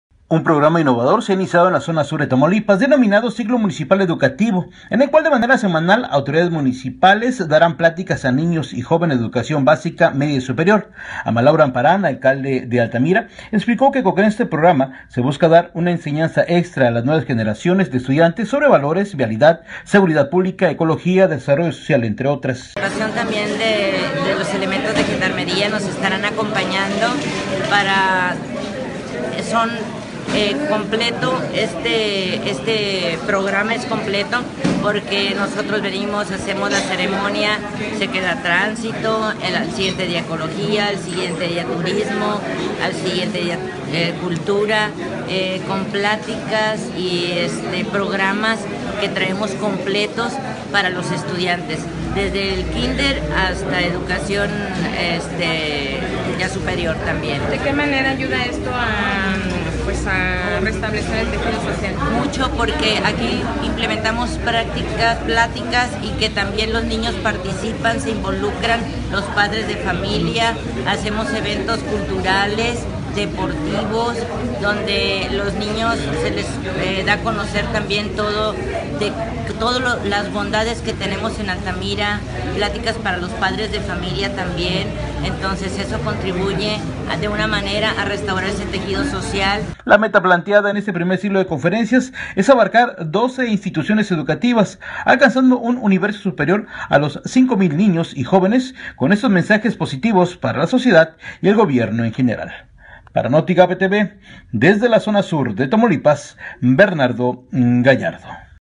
Autoridades enfocadas en fortalecer tejido social
Un programa innovador se ha iniciado en la zona sur de Tamaulipas, denominado "ciclo municipal educativo", en el cual de manera semanal, autoridades municipales de diferentes direcciones, apoyados con tránsito, gendarmería, y otras autoridades, llevarán diferentes pláticas a niños y jóvenes de educación básica, media y superior.